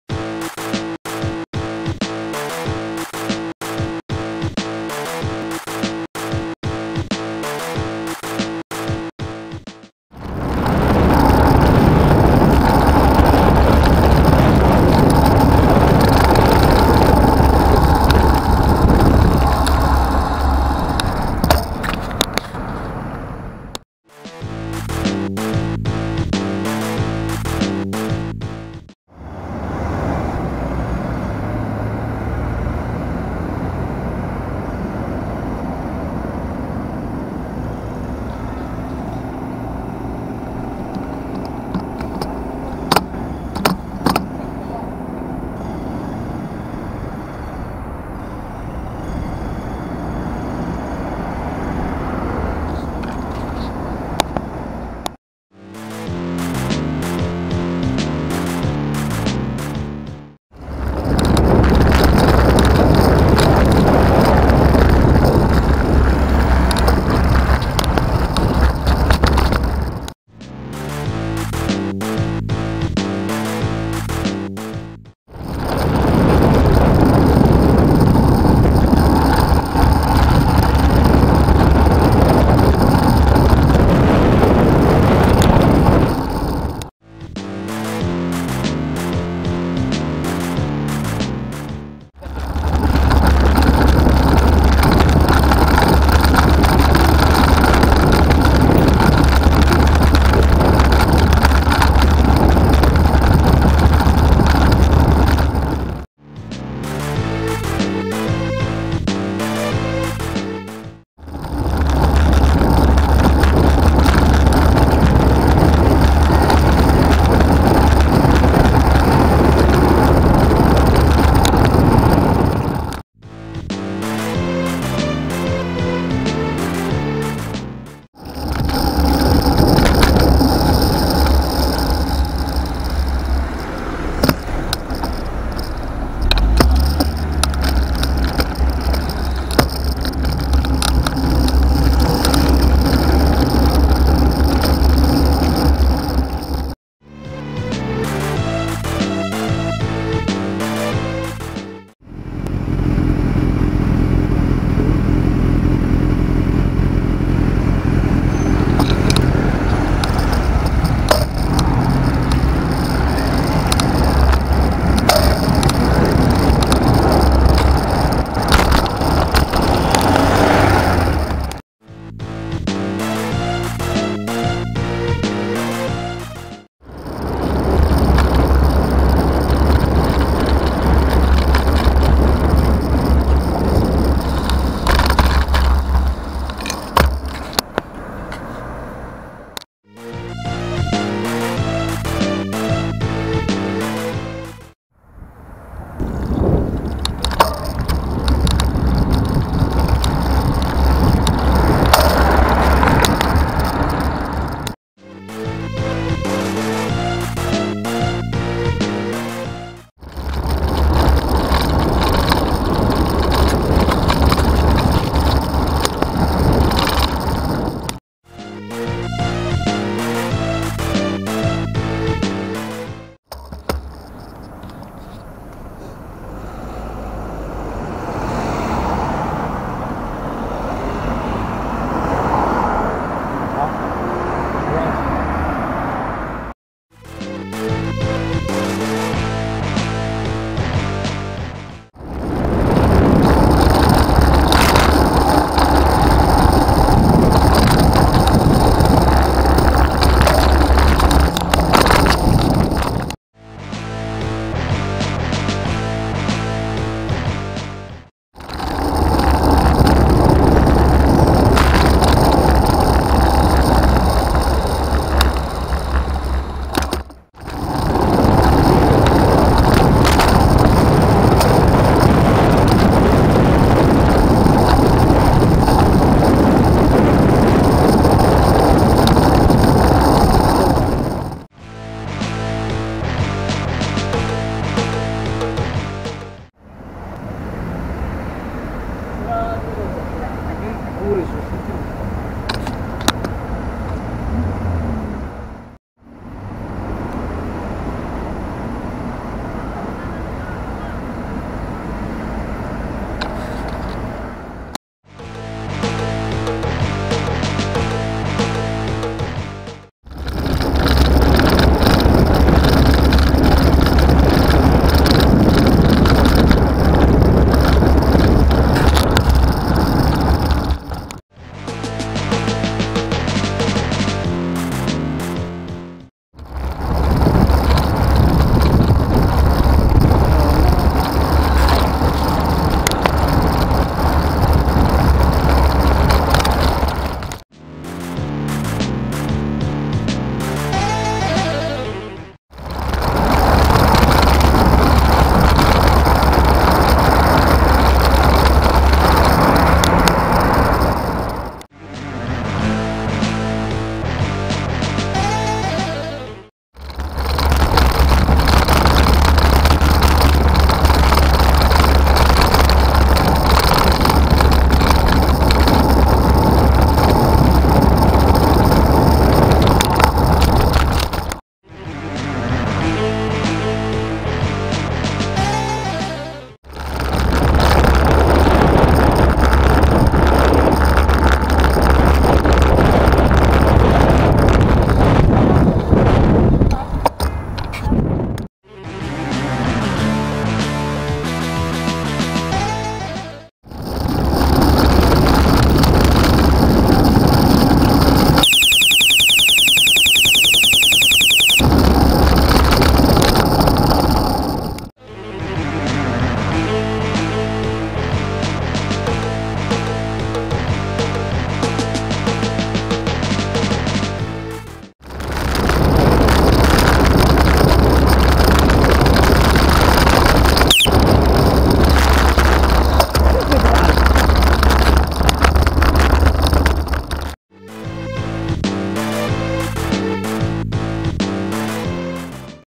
Cycle "Adventures" Volume 34
Quite a long one this time. Lots of mini incidents in a short time.

Yes, I'm well aware the date is out by a month on many of these clips ;)